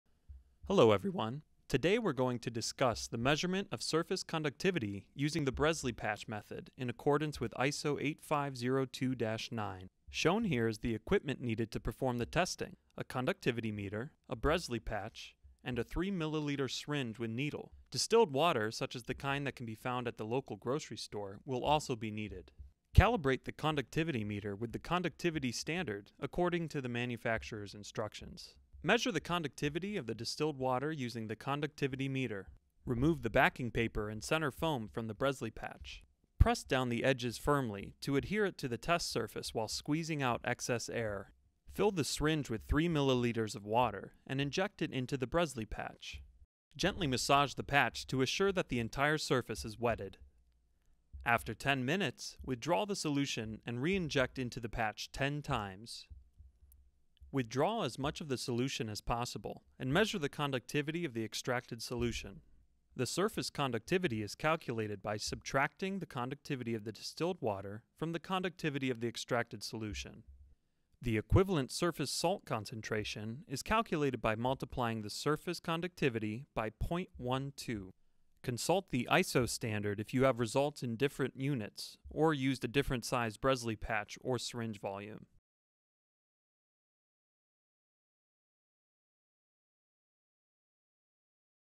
Measurement of Surface Conductivity Using the Bresle Patch Method
Today we're going to discuss the measurement of surface conductivity using the Bresle patch method in accordance with ISO 8502-9

Browse KTA's Bresle Patch Kit!

Browse Surface Contamination Test Kits (SCAT) here!